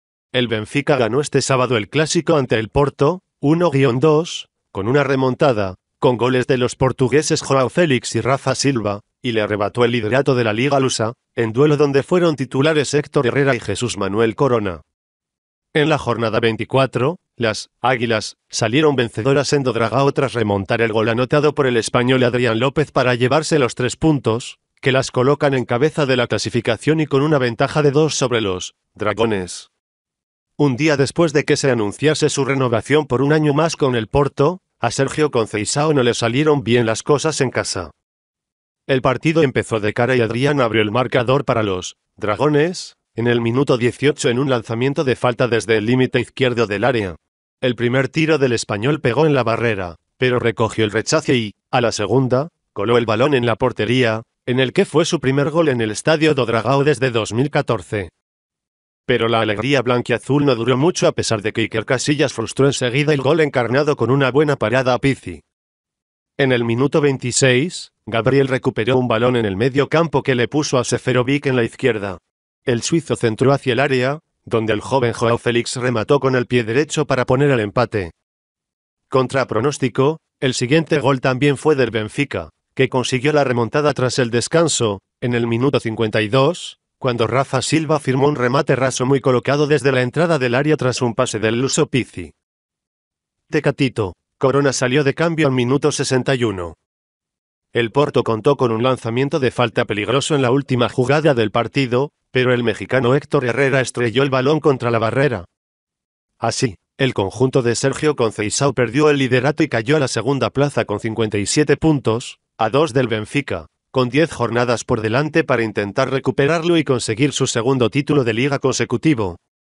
Héctor Herrera y Jesús 'Tecatito' Corona titulares en derrota del Porto 1-2 ante el Benfica
El Benfica ganó este sábado el Clásico ante el Porto (1-2) con una remontada, con goles de los portugueses João Félix y Rafa Silva, y le arrebató el liderato de la Liga lusa, en duelo donde fueron titulares Héctor Herrera y Jesús Manuel Corona.

En la jornada 24, las "águilas" salieron vencedoras en Do Dragão tras remontar el gol anotado por el español Adrián López para llevarse los tres puntos, que las colocan en cabeza de la clasificación y con una ventaja de dos sobre los "dragones".

Un día después de que se anunciase su renovación por un año más con el Porto, a Sérgio Conceição no le salieron bien las cosas en casa.

El partido empezó de cara y Adrián abrió el marcador para los "dragones" en el minuto 18 en un lanzamiento de falta desde el límite izquierdo del área. El primer tiro del español pegó en la barrera, pero recogió el rechace y, a la segunda, coló el balón en la portería, en el que fue su primer gol en el Estadio do Dragão desde 2014.

Pero la alegría blanquiazul no duró mucho a pesar de que Iker Casillas frustró enseguida el gol encarnado con una buena parada a Pizzi.

En el minuto 26, Gabriel recuperó un balón en el medio campo que le puso a Seferovic en la izquierda. El suizo centró hacia el área, donde el joven João Félix remató con el pie derecho para poner el empate.

Contra pronóstico, el siguiente gol también fue del Benfica, que consiguió la remontada tras el descanso, en el minuto 52, cuando Rafa Silva firmó un remate raso muy colocado desde la entrada del área tras un pase del luso Pizzi.

‘Tecatito’ Corona salió de cambio al minuto 61.

El Porto contó con un lanzamiento de falta peligroso en la última jugada del partido, pero el mexicano Héctor Herrera estrelló el balón contra la barrera.

Así, el conjunto de Sérgio Conceição perdió el liderato y cayó a la segunda plaza con 57 puntos, a dos del Benfica, con diez jornadas por delante para intentar recuperarlo y conseguir su segundo título de Liga consecutivo.